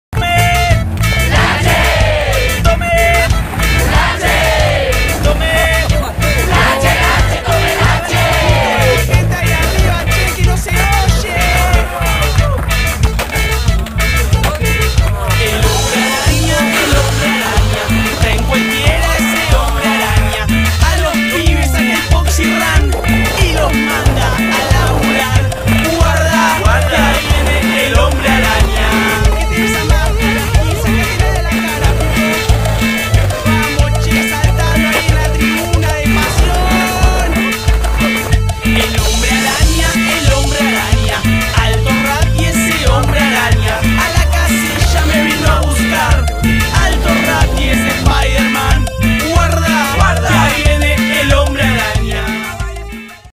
MungaVideo - Trailer #1
Una serie de videos inspirados en chocar los 5, diversion y boludear todo el dia.